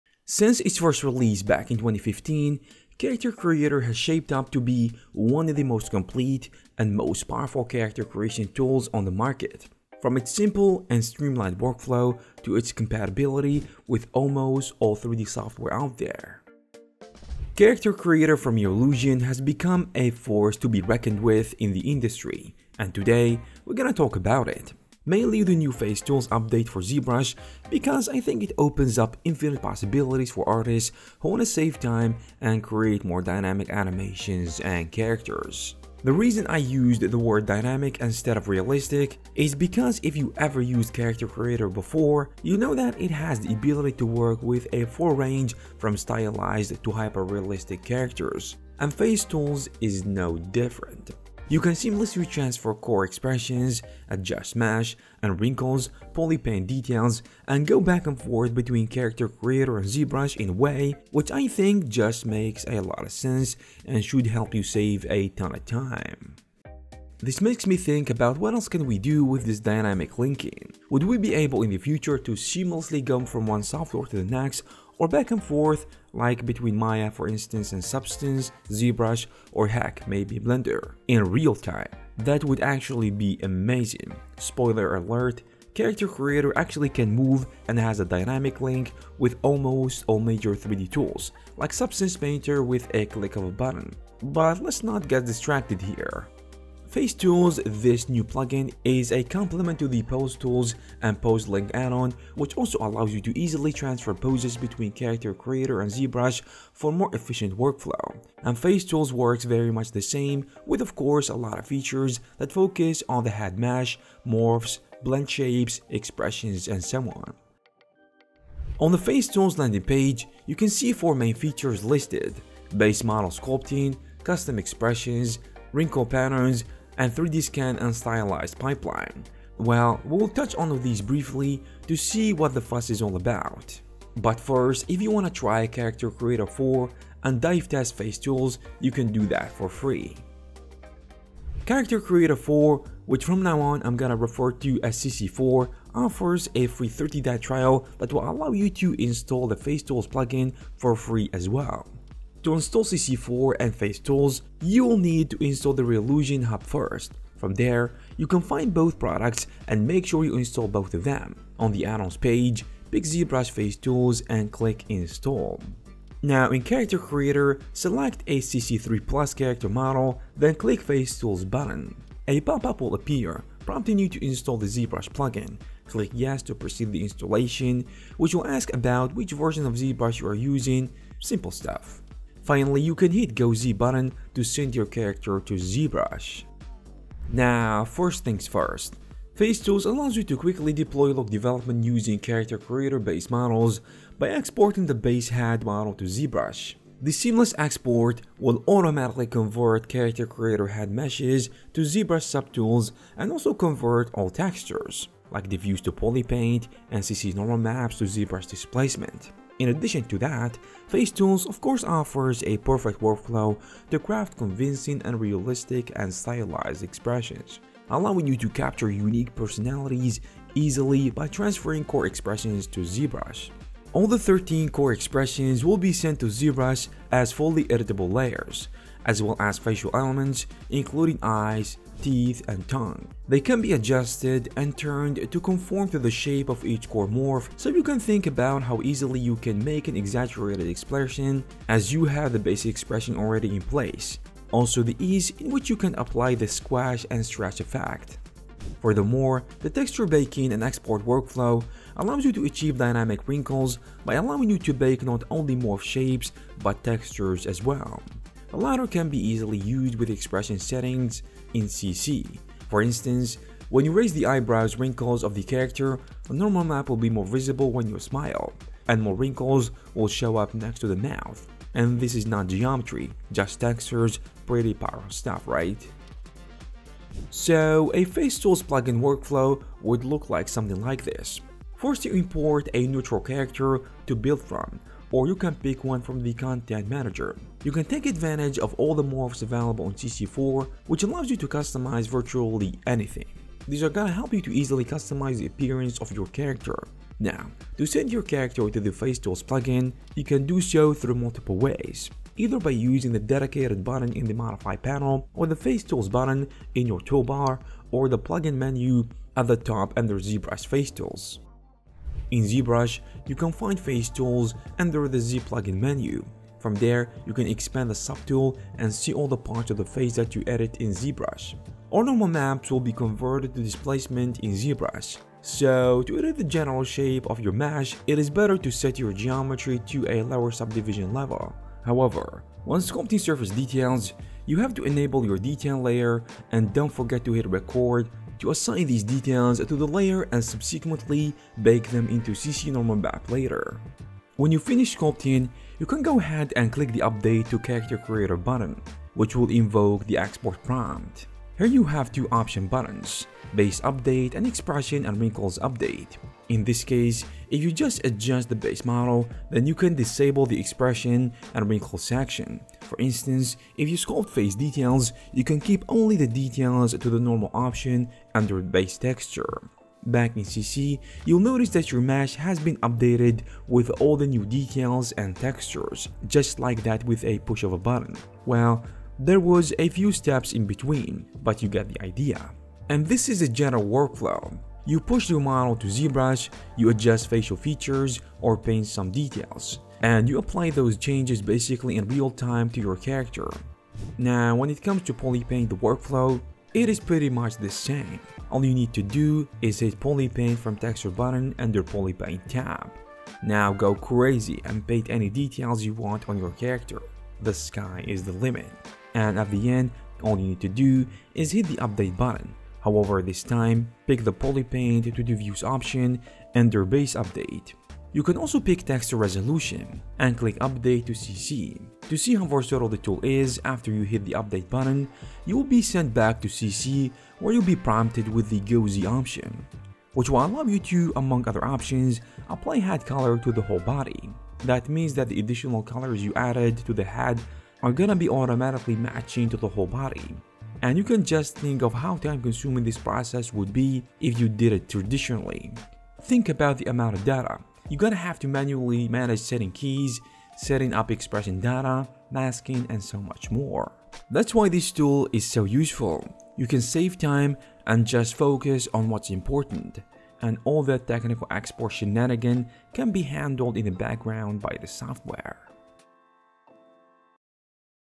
Can This 3D Tool Make That Much Difference?!!
Check out Character Creator, the go-to for creating awesome characters since 2015. And now, with the FaceTools update, it's a game-changer for artists, letting you make cooler animations and characters in a snap.

Productivity►:
Clean Panels